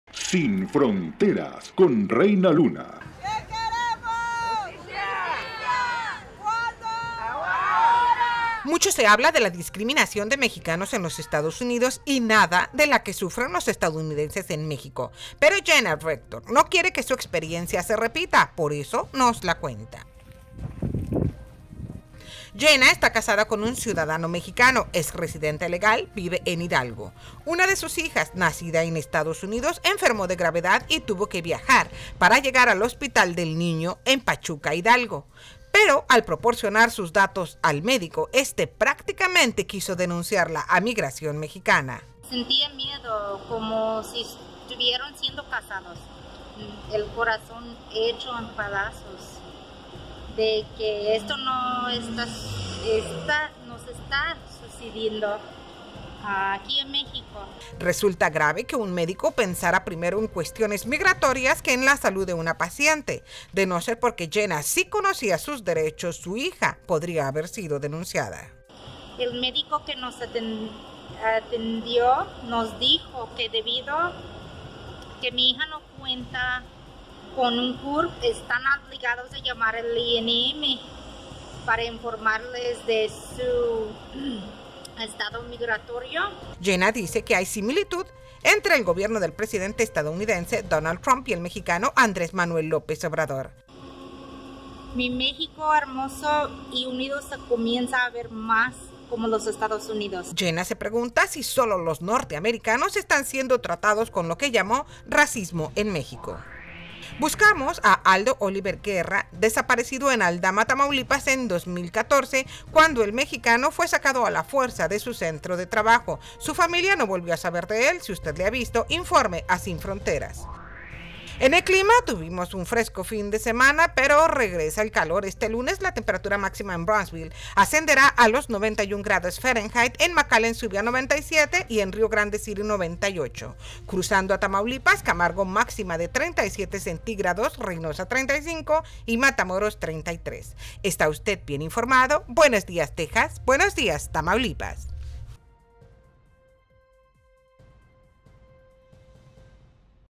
Médico mexicano más interesado en el estado migratorio de una paciente que en su salud
Jenna Rector, residente mexicana,  llevó a su niña nacida en Estados Unidos al médico en Hidalgo... y el médico antes de ver por la salud de su paciente, vió su estado migratorio!!! Buscamos a Aldo Oliver Guerra, mexicano desaparecido en Aldama, Tamaulipas... En el clima, se nos va el fresco y regresa el calor...